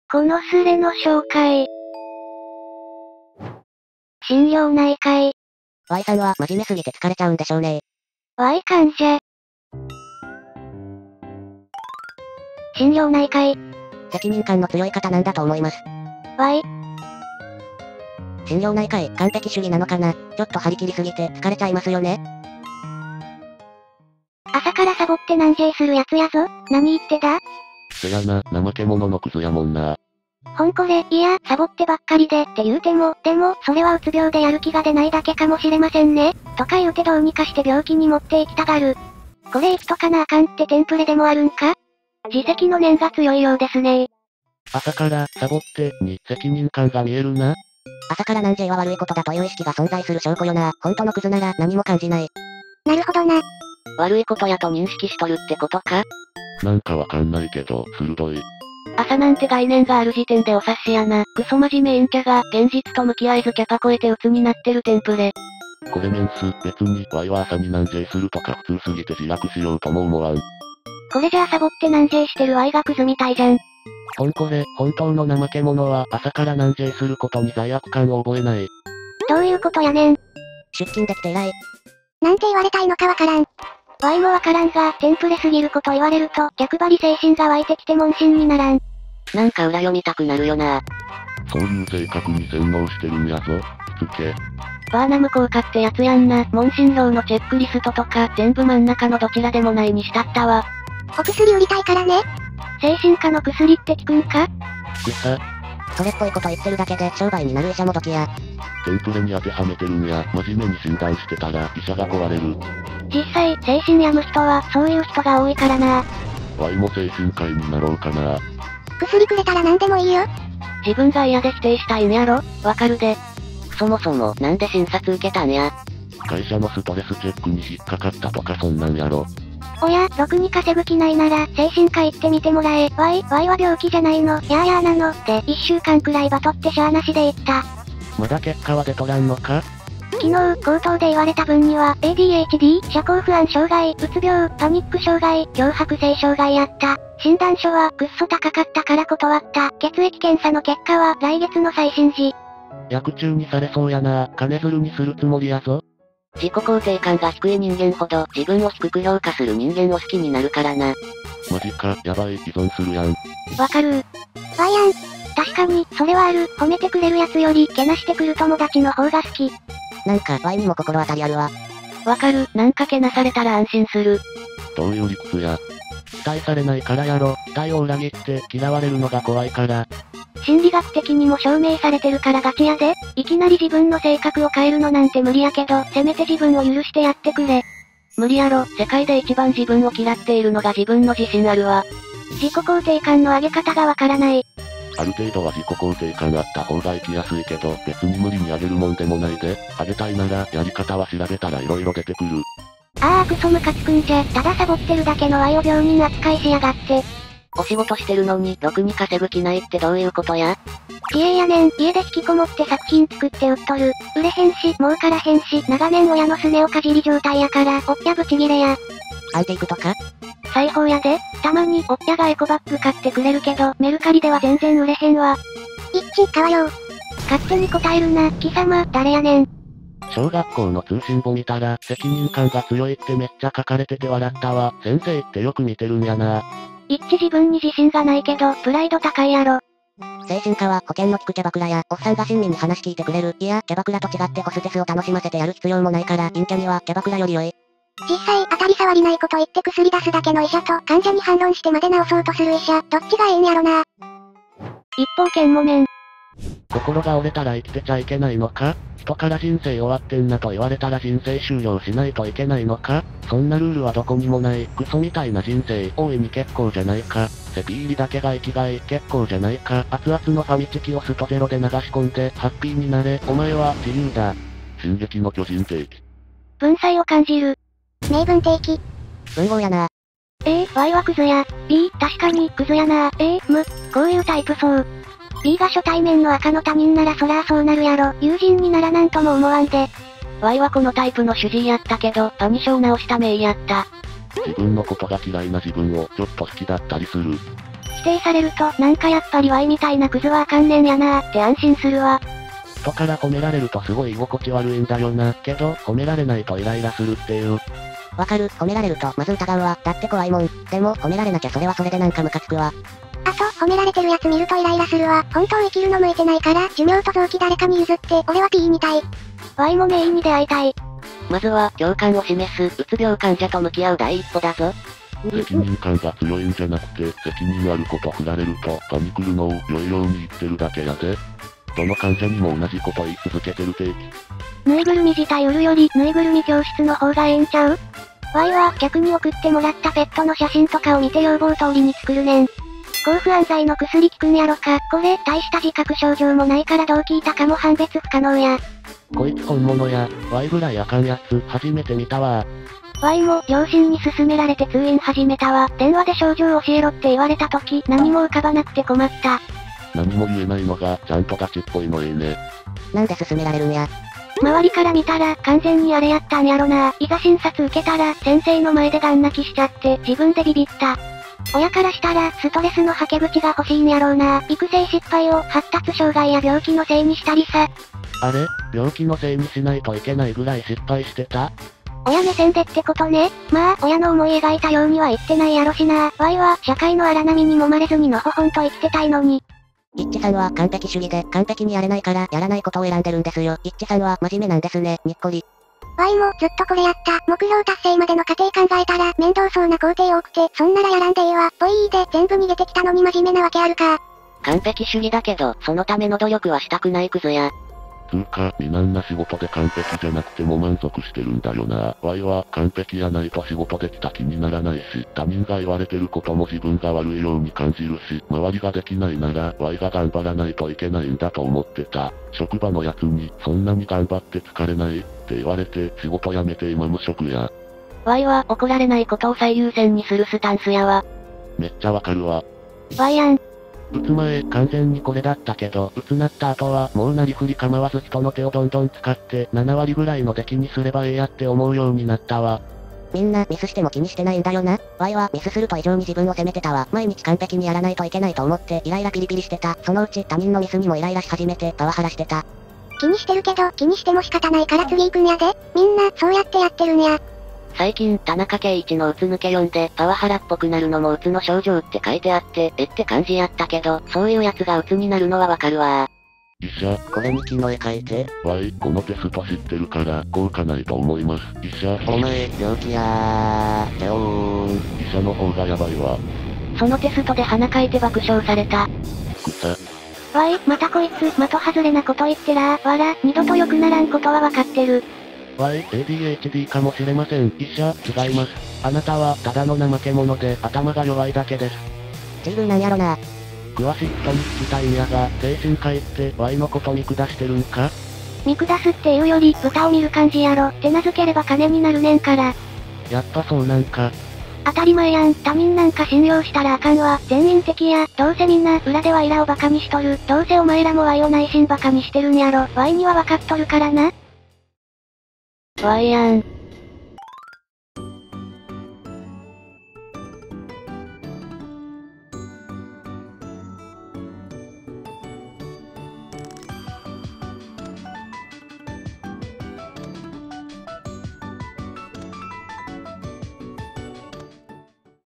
【医療ミス】ワイ、ニートなのに心療内科医に「真面目で病みやすい」と言われる…【2ch面白いスレ】
2ch面白いスレを紹介！

暖かいコメントがほんと励みになります😭

いらすとや 様 , 効果音ラボ 様 , OtoLogic 様
Youtuberのための素材屋さん 様 , daidaicolor 様
その他素材提供者様, ありがとうございます！

元スレッド：2ch.sc様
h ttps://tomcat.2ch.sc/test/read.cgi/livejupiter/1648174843